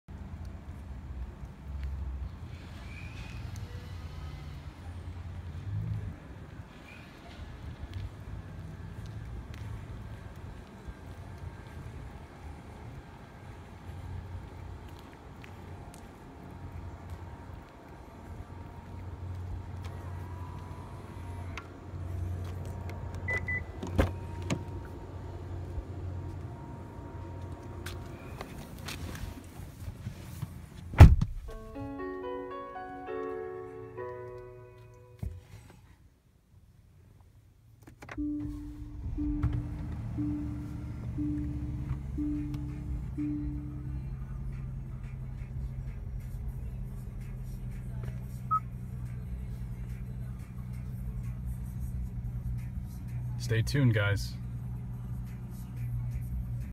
My New Genesis Reveal Teaser
I finally rejoin the Genesis family with the addition of my all new Genesis sedan.  It is Casablanca White with Ultimate package.  This is the 3.8L V6 with AWD (for the bad NE winters). I almost opted for the 5.0 but I feel like the V6 has plenty of power still.  I traded my base 2014 WRX for an insanely loaded car and couldn't be happier.  Many mods in her future including springs, wheels, and upgraded rotors and pads.